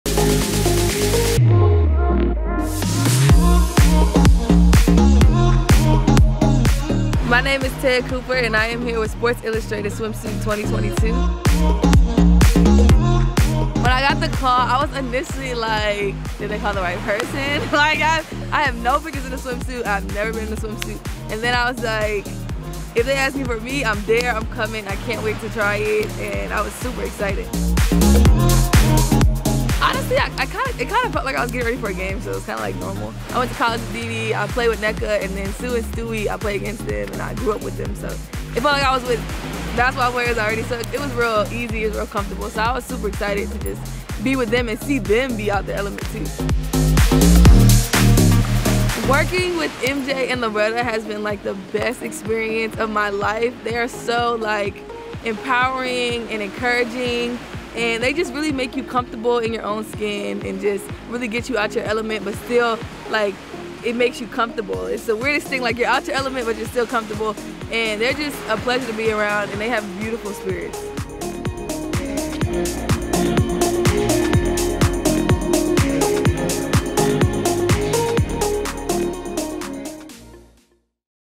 Highlights of Te’a Cooper’s 2022 Photo Shoot in St. Thomas
Te’a Cooper was photographed in St. Thomas by Laretta Houston shooting for SI Swimsuit 2022.

“Working with MJ and Loretta has been the best experience of my life. They’re so empowering and encouraging. They just really make you comfortable in your own skin… It’s the weirdest thing you’re out of your element but you’re still comfortable. They’re just a pleasure to be around and they have beautiful spirits.”

Follow SI Swim on social!